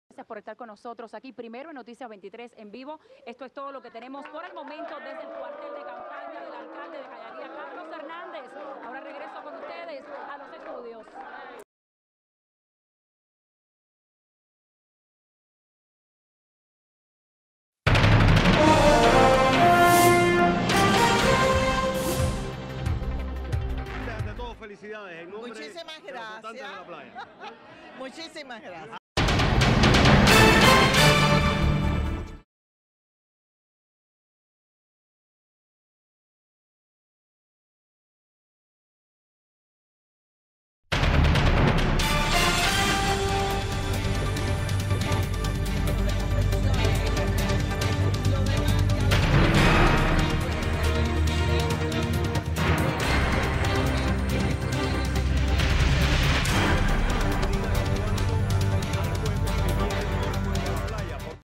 Election Mic Snatching & Name Fail
Set of two news "bloopers". First one someone random decides to try and snatch the microphone away from the reporter. The other is pretty much a cardinal sin in the news business, a mispelled name. Not just mispelled, but descriptive of a bodily function.